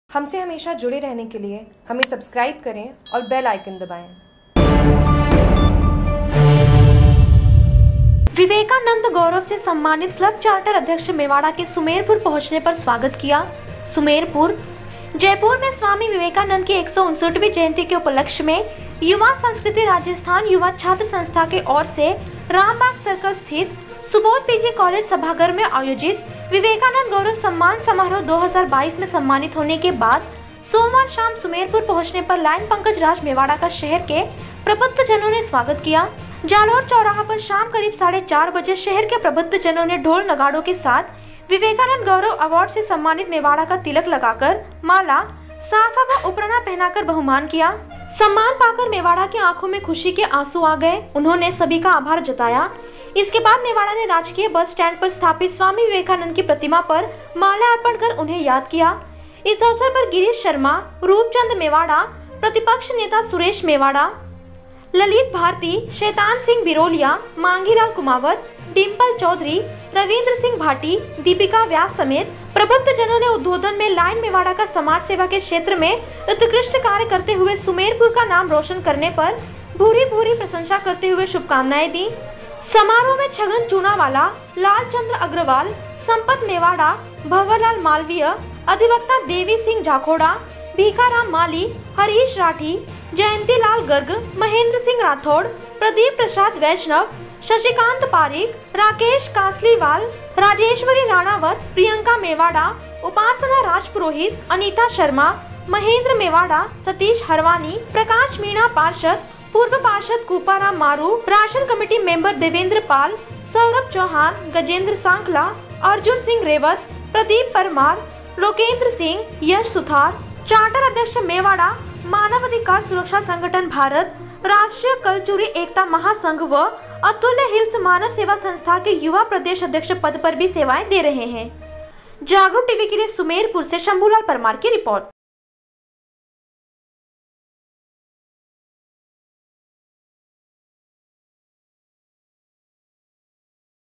विवेकानन्द गाैरव से सम्मानित क्लब चार्टर अध्यक्ष मेवाड़ा के सुमेरपुर पंहुचने पर स्वागत किया
सुमेरपुर। जयपुर में स्वामी विवेकानंद की 159वीं जयंती के उपलक्ष में युवा संस्कृति राजस्थान युवा छात्र संस्था की ओर से रामबाग सर्कल स्थित सुबाेध पीजी काॅलेज सभागार में अायाेजित विवेकानंद गाैरव सम्मान समाराेह-2022 में सम्मानित हाेने के बाद साेमवार शाम सुमेरपुर पंहुचने पर लाॅयन पंकज राज मेवाड़ा का शहर के प्रबुद्धजनाें ने स्वागत किया। जालाेर चाैराह पर शाम करीब 4.30 बजे शहर के प्रबुद्धजनाें ने ढाेल-नगाड़ाे के साथ विवेकानंद गाैरव अवार्ड से सम्मानित मेवाड़ा का तिलक लगाकर, माला, साफा व उपरना पहनाकर बहुमान किया। सम्मान पाकर मेवाडा के अांखाे में खुशी के अांसू अा गए। उन्हांेने सभी का अाभार जताया। इसके बाद मेवाड़ा ने राजकीय बस स्टेण्ड पर स्थापित स्वामी िववेकानंद की प्रतिमा पर माल्यार्पण कर उन्हंे याद िकया।